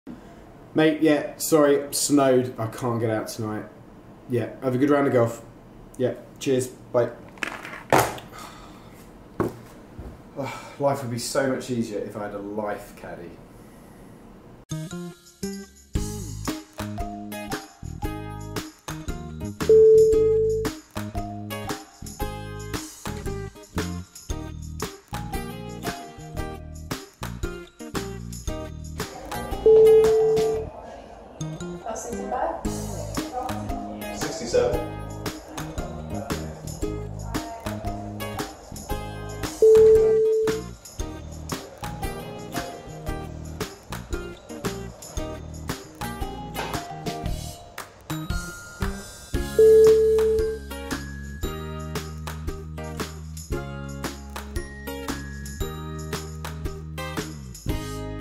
Paige Spiranac's life caddy
On the golf course a caddy can be an invaluable resource. Imagine if we could factor one into every day life... Well, thanks to the help of Social Media hotshot Paige Spiranac we don't have to imagine.